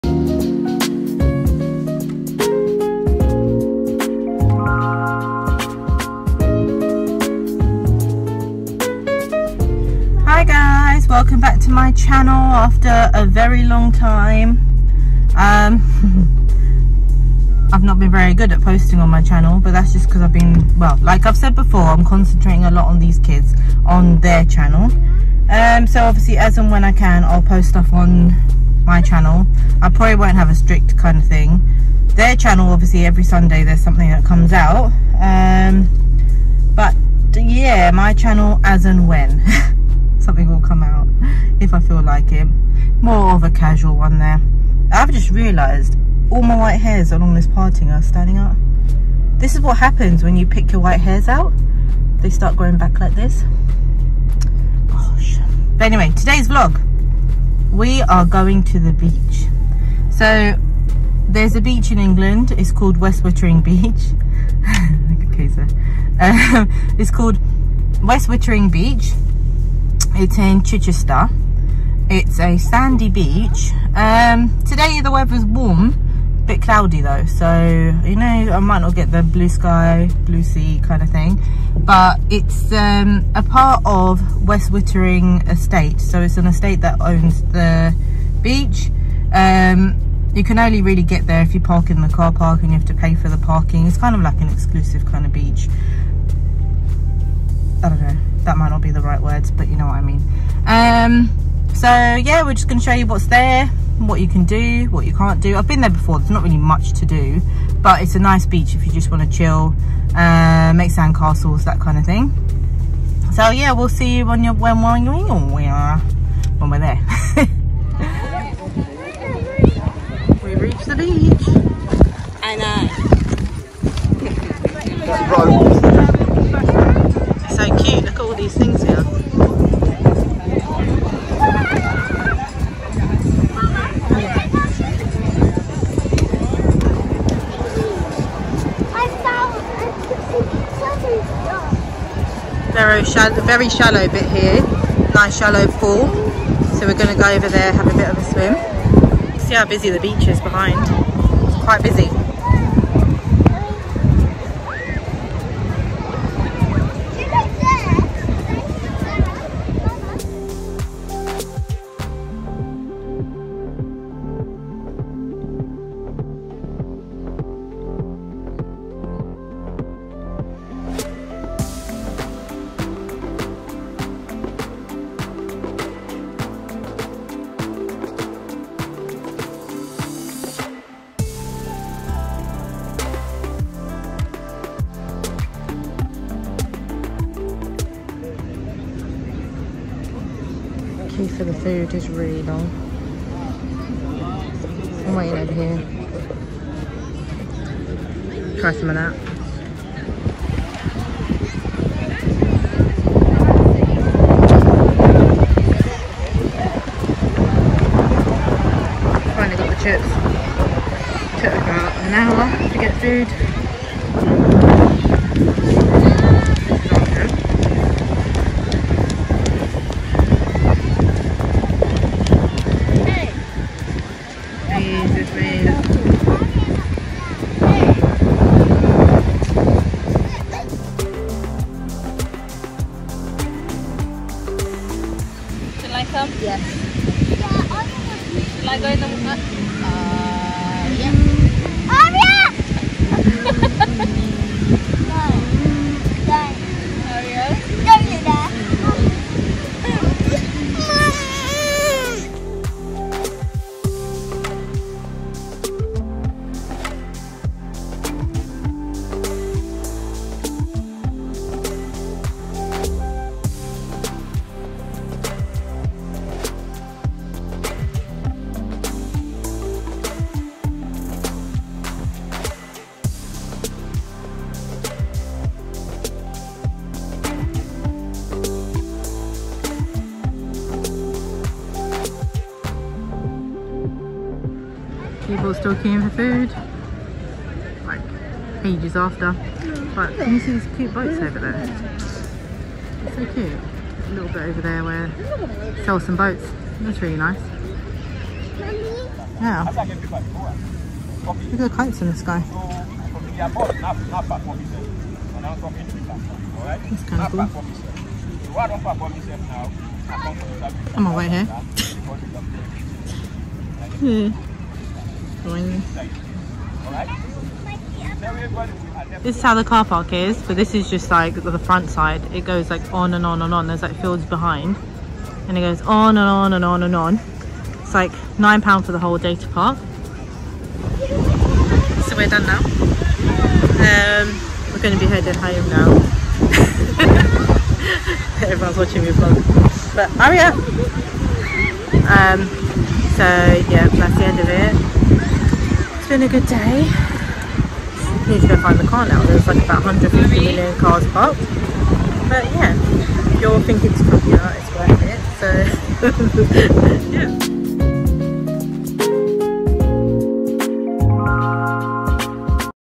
We Visit West Wittering Beach 🌊
We visited West Wittering Beach. Was such a lovely day! The sun was out and the sea was warm and clean. Such a beautiful beach to visit!